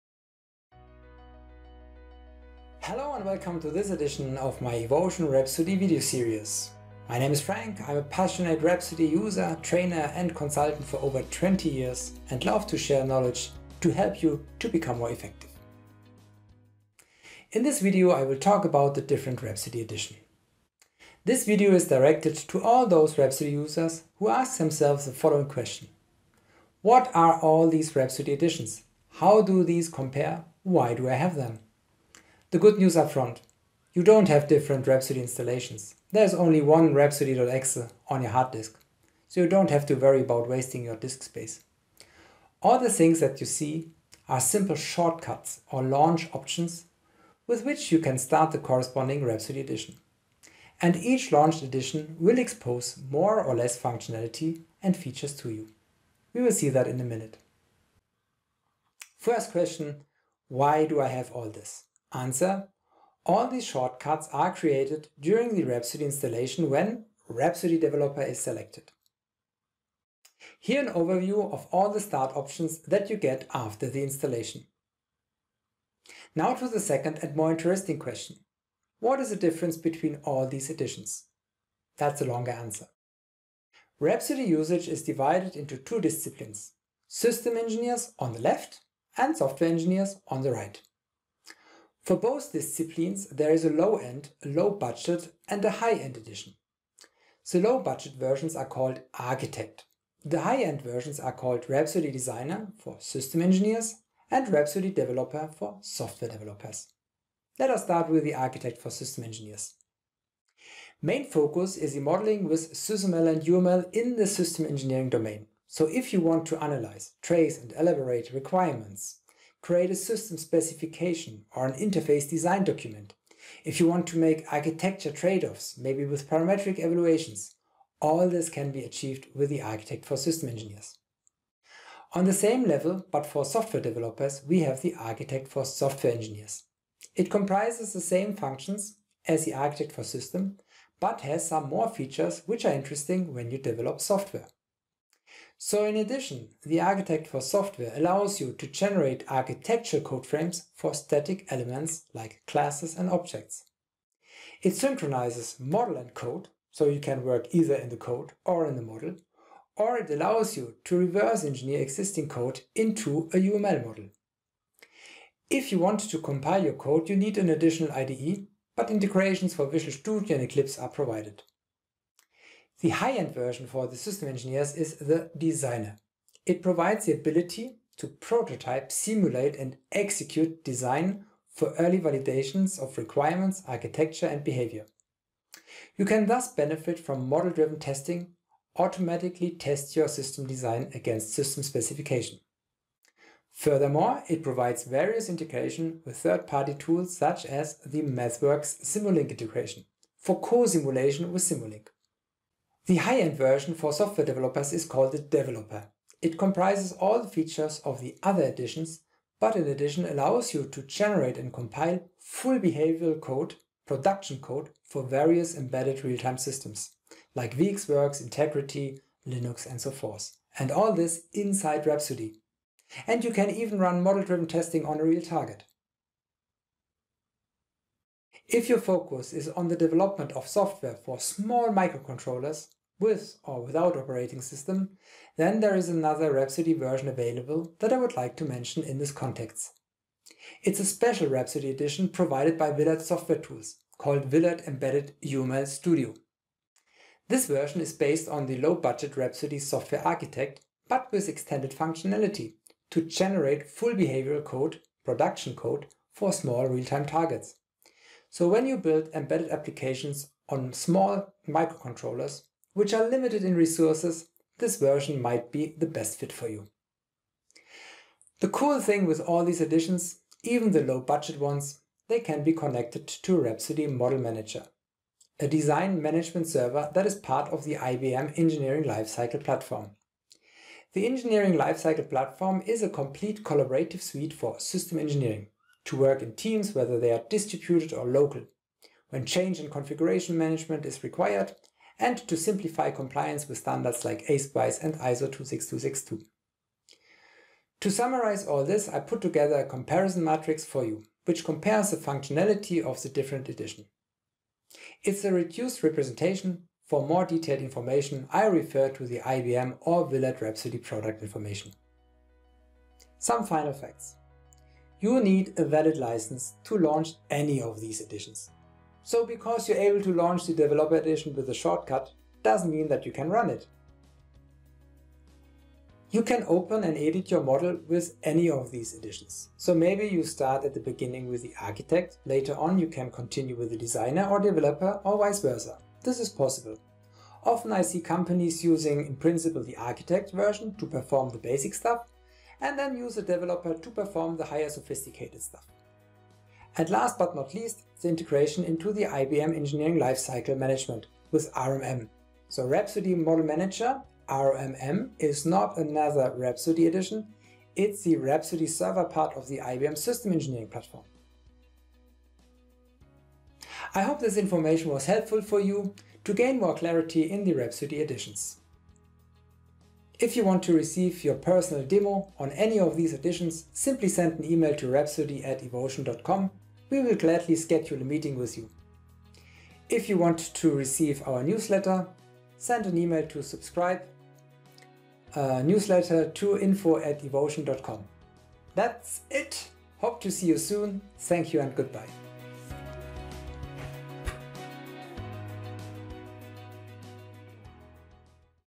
[IBM-Rhapsody] Comparison of the different Rhapsody Editions - incl. [Willert Embedded UML Studio]
If you ever asked yourself the question: What is the difference between all these different IBM-Rhapsody editions? (Rhapsody Architect for Systems Engineers , Rhapsody Architect for Software Engineers, Rhapsody Developer, Rhapsody Designer).
In this Video I give an overview of the different Rhapsody Edition and will also  talks about the [Willert-Embedded Studio] and the [IBM Engineering Lifecycle Platform] with Rhapsody Model Manager.

Enjoy this video?  Give it a like :)